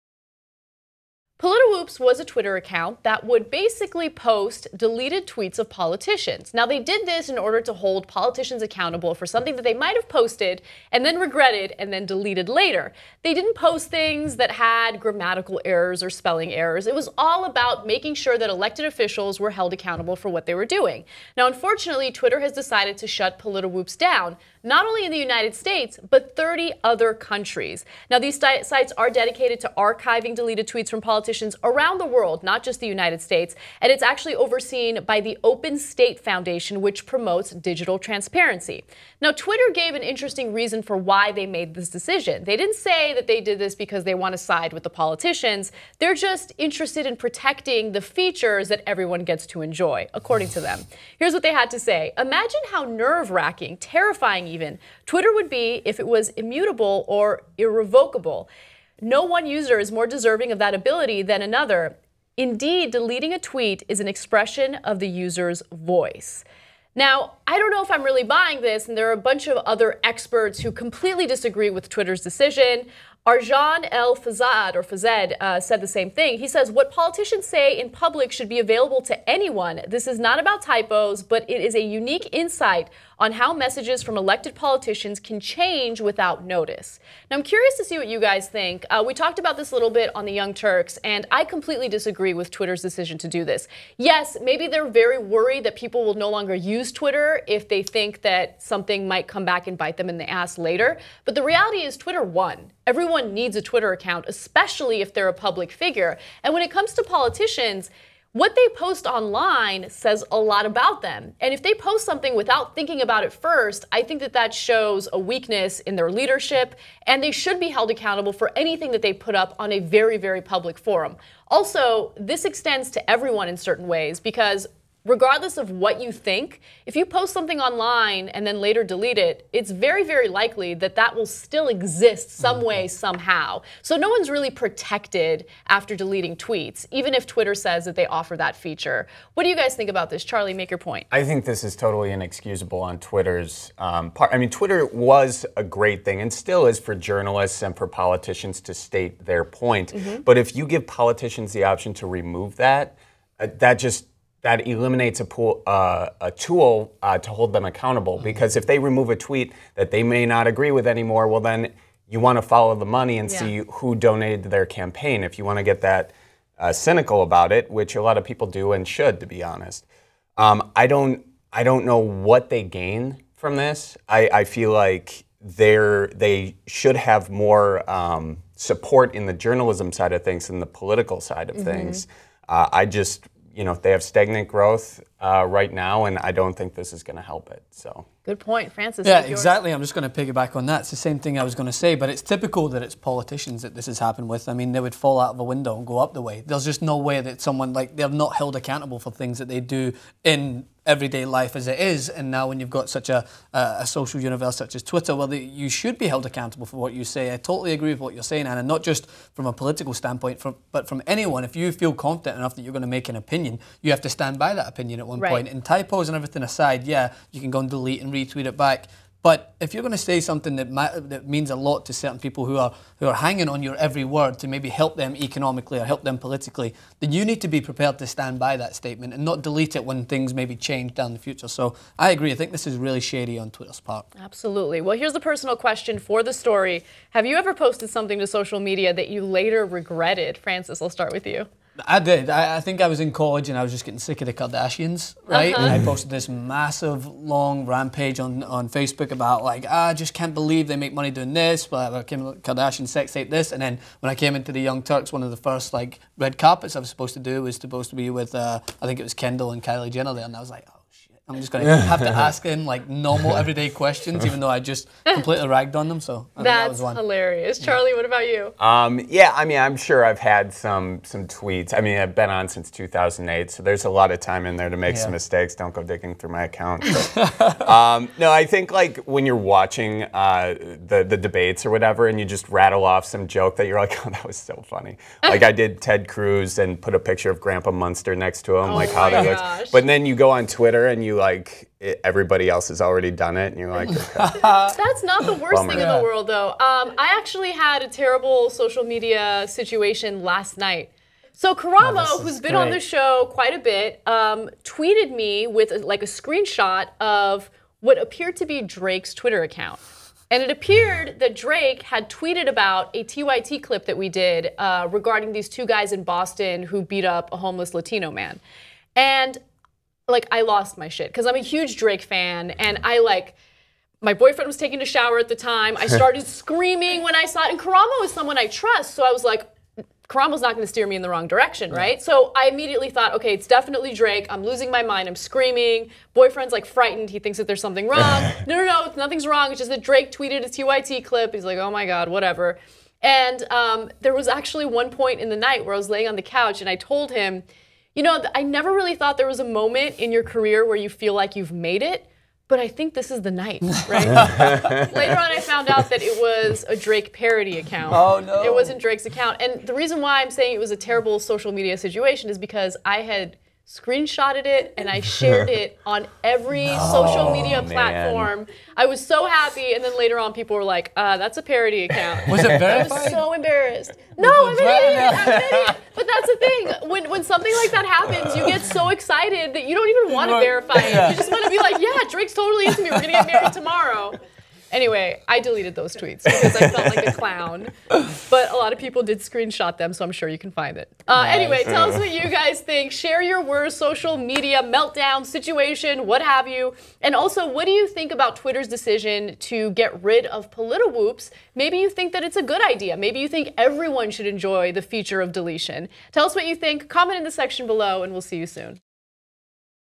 Twitter Defending IDIOT Politicians!
The social media site has suspended 30 sites, collectively known as Politwoops and Diplotwoops, that are dedicated to archiving deleted tweets from politicians around the world.

Have you ever posted something to a social media site that you later regretted? Comment in the section below!

Host:
Ana Kasparian

Guests:
Charlie Berens